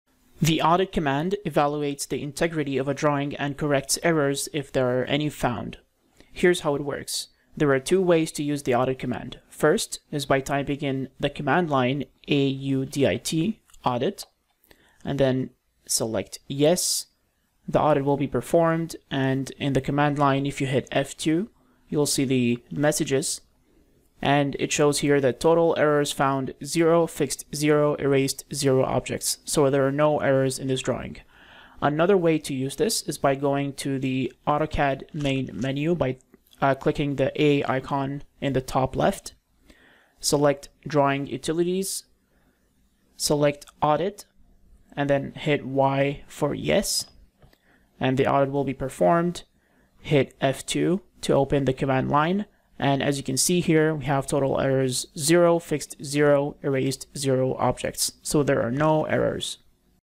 Evaluate the integrity and errors of a drawing | AUDIT | AutoCAD Tips in 60 Seconds
The AUDIT command allows you to evaluate the integrity and correct errors in a drawing. This is mainly helpful in larger drawings where it has been used by many users and modified intensively. It is good practice to regularly audit drawings to ensure that they keep running smoothly and does not cause a crash. The AUDIT command will erase objects that cause errors.